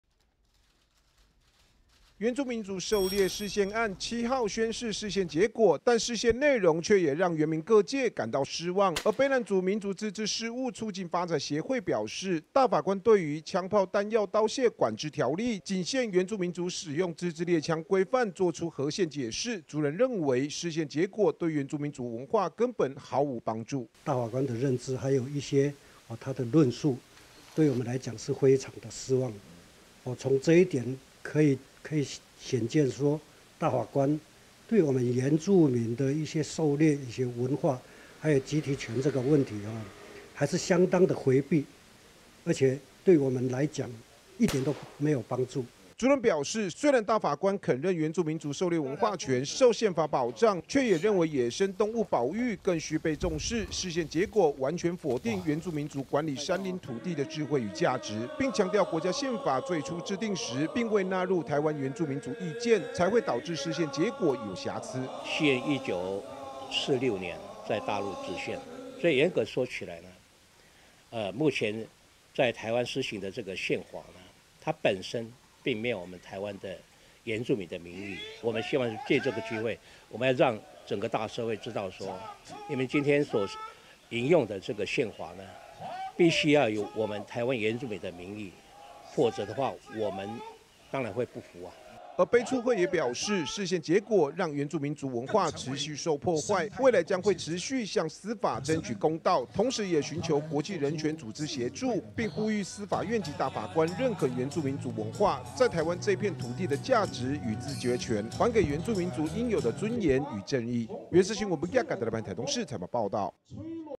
對狩獵釋憲案感失望 卑促會籲肯認原民文化 2021-05-14 IPCF-TITV 原文會 原視新聞
原住民族狩獵釋憲案7號宣示釋憲結果，但釋憲內容卻也讓原民各界感到失望，而卑南族民族自治事務促進發展協會表示，大法官對於"槍砲彈藥刀械管制條例"，僅限原住民使用自製獵槍規範，做出"合憲"解釋，族人認為，釋憲結果對原住民族文化根本毫無幫助。

族人表示，雖然大法官肯認原住民族狩獵文化權受憲法保障，卻認為野生動物保育更需被重視，釋憲結果完全否定原住民族管理山林土地的智慧與價值，並強調國家憲法最初制定時，並未納入台灣原住民族意見，才導致釋憲結果有瑕疵。

而卑促會也表示，釋憲結果讓原住民族文化持續受迫害，未來將會持續向司法爭取公道，同時也尋求國際人權組織協助，並呼籲司法院及大法官認肯原住民族文化，在台灣這片土地的價值與自決權，還給原住民族應有的尊嚴與正義。